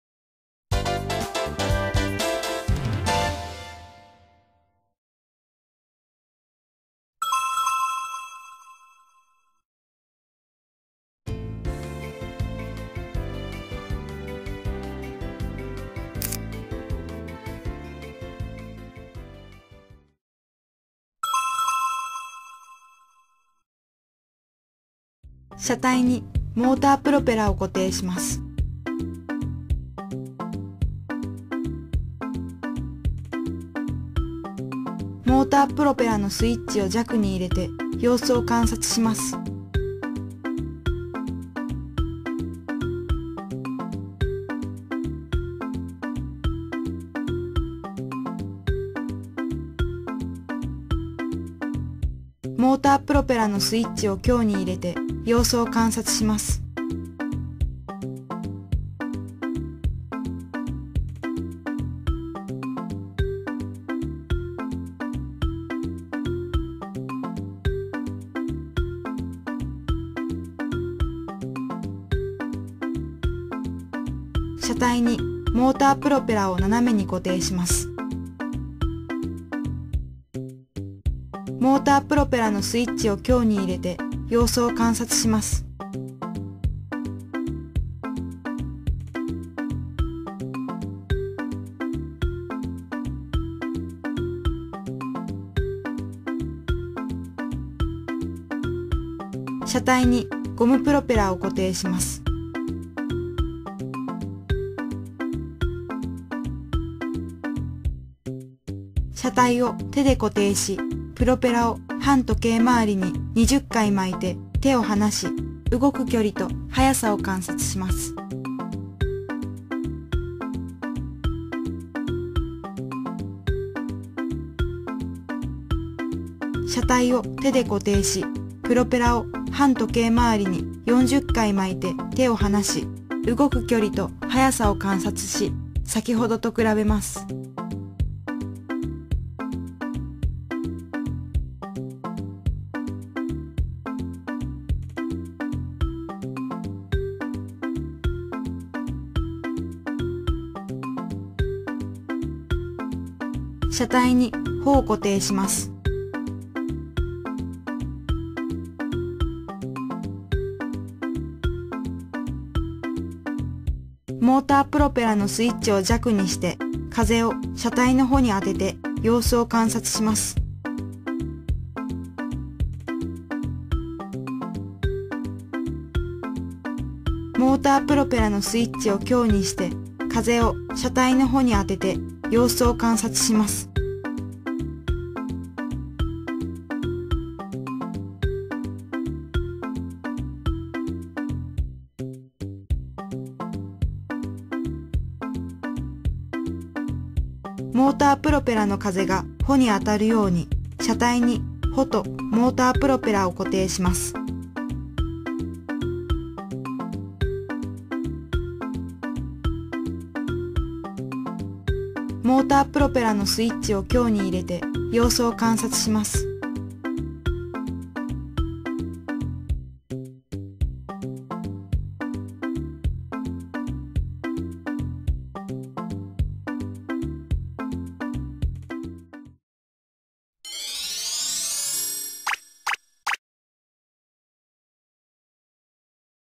実験レシピ プロペRUNを風の力で動かそう！
風やゴムの力でものを動かすことができることを確かめよう！
(Let's start getting Puroperan wind power!)